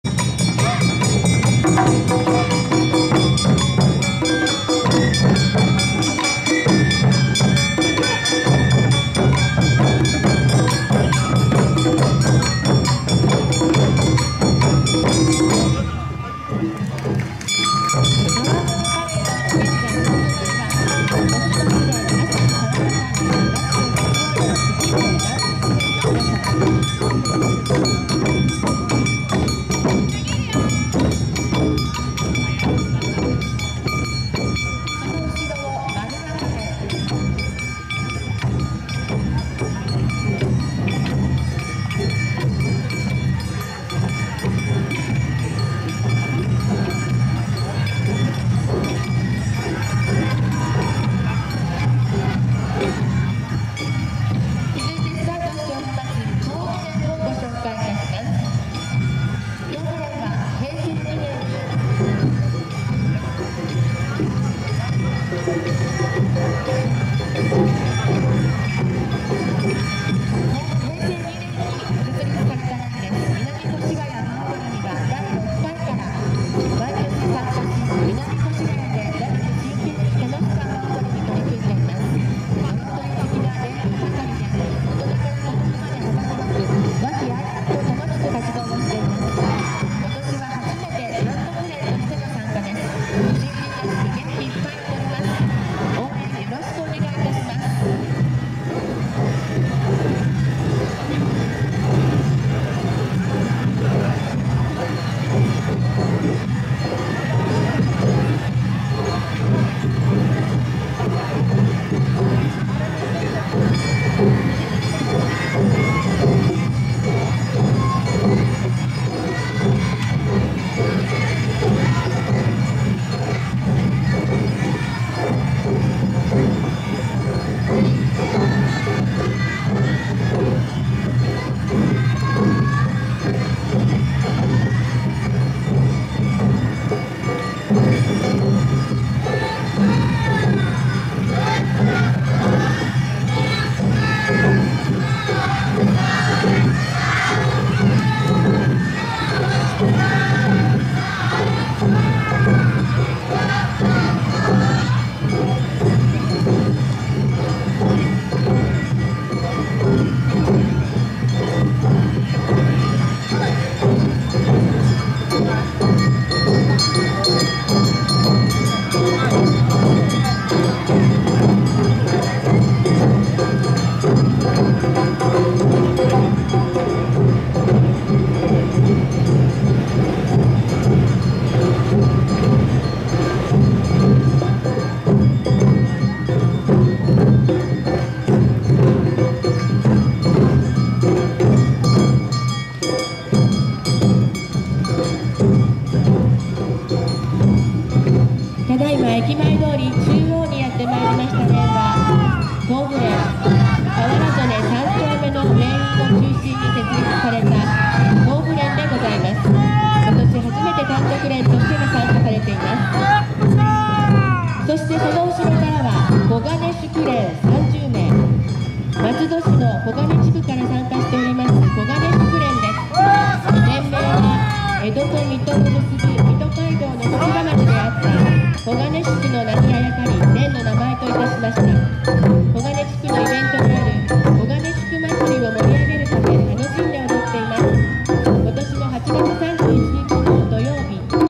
東武連／2019 堀切かつしか菖蒲まつりパレード
2019/06/09（令和元年6月9日）
【祭り】2019葛飾菖蒲まつり
【場所】京成線「堀切菖蒲園」駅前通り